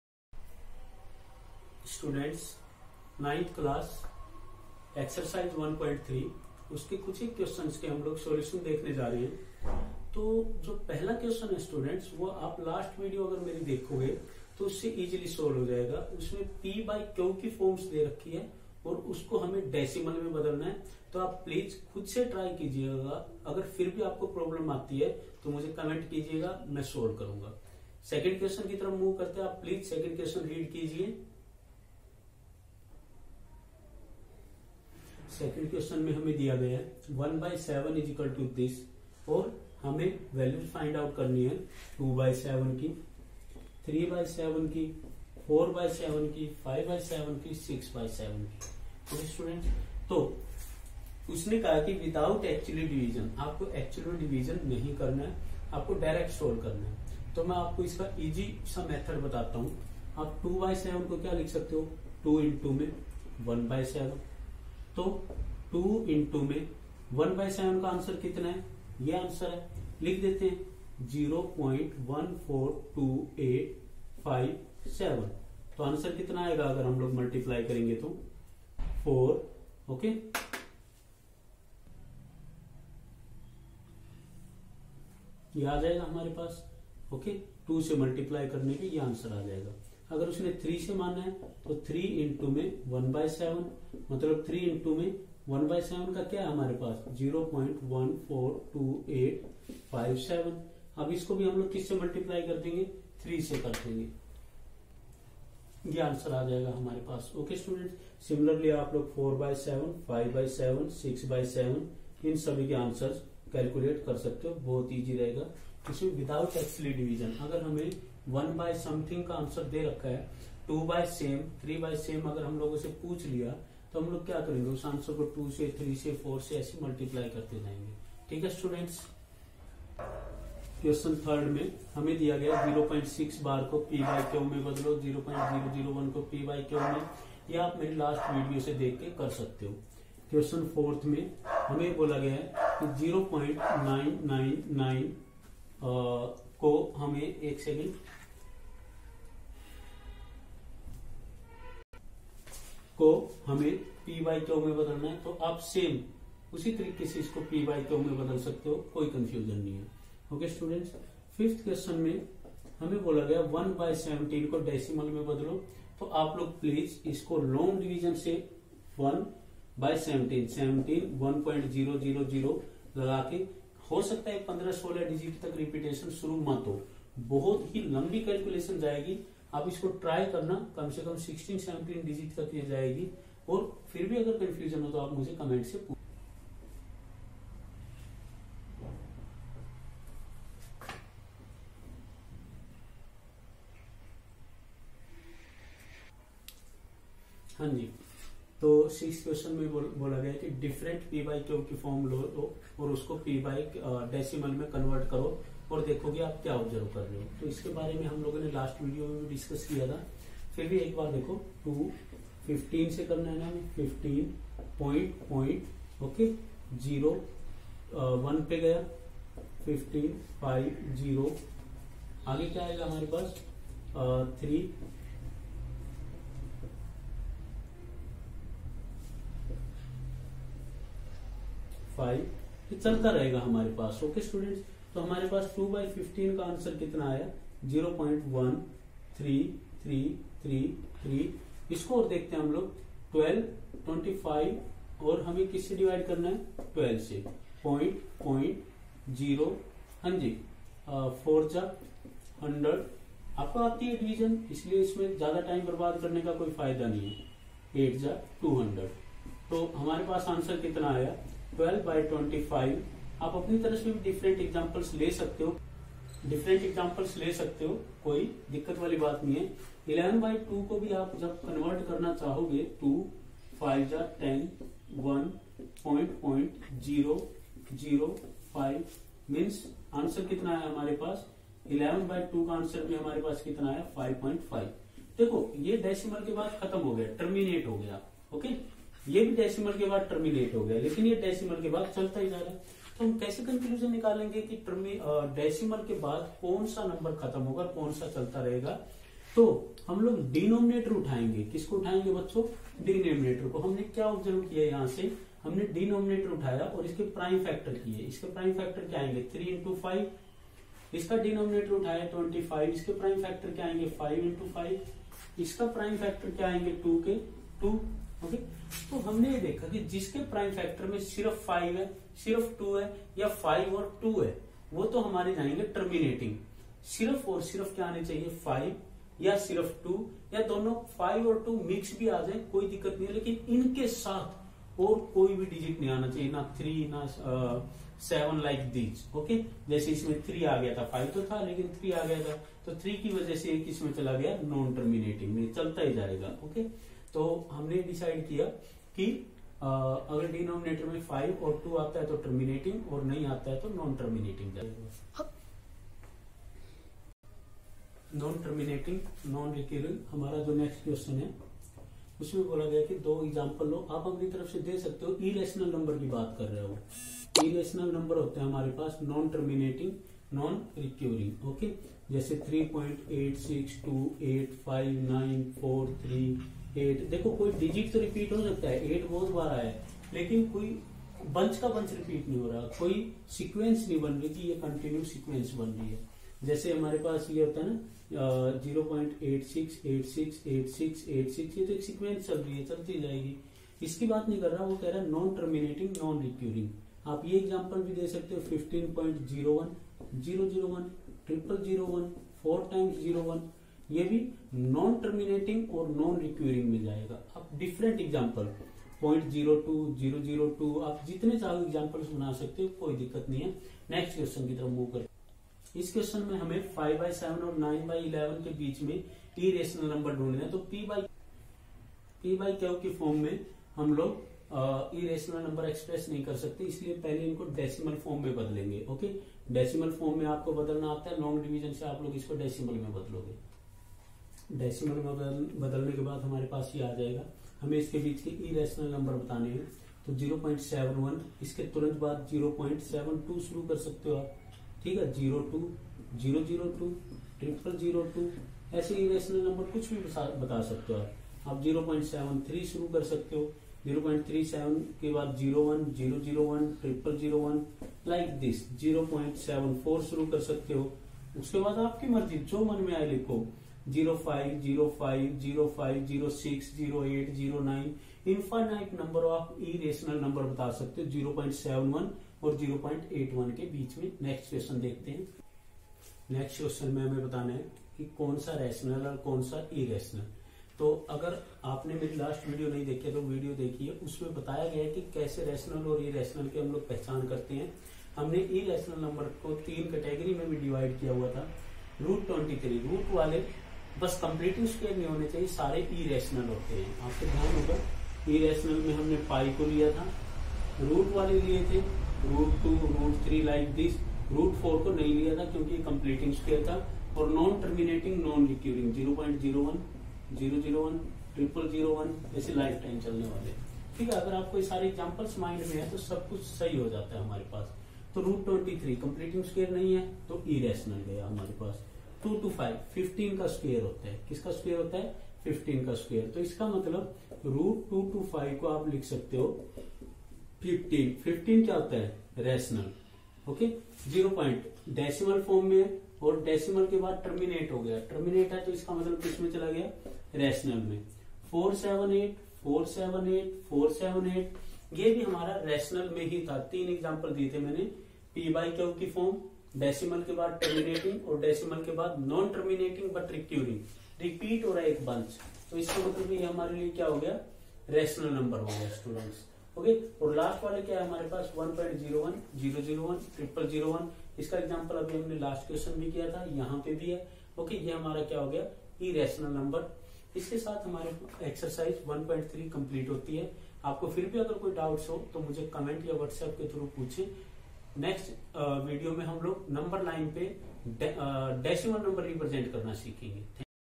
Maths 9 Ex 1.3 p by q to decimal, decimal to p by q
By Jandeep Sir